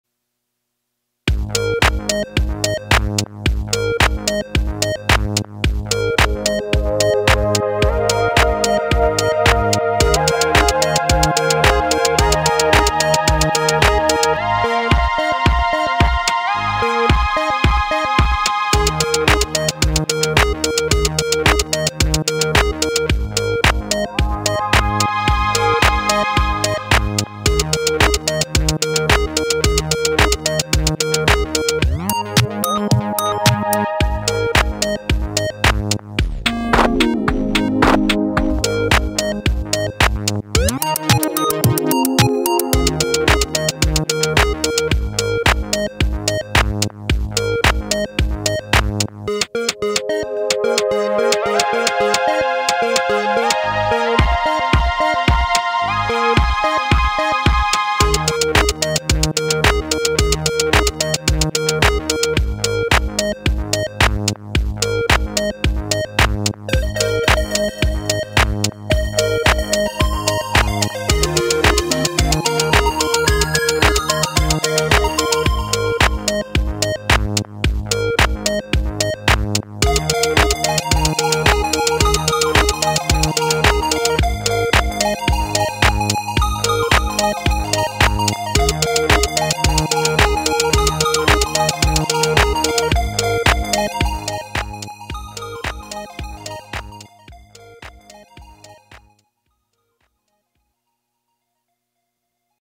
PO-33 + Kaossilator jam
KO2 combined with the Pocket Operator PO-33 micro-sampler opens up so many possibilities.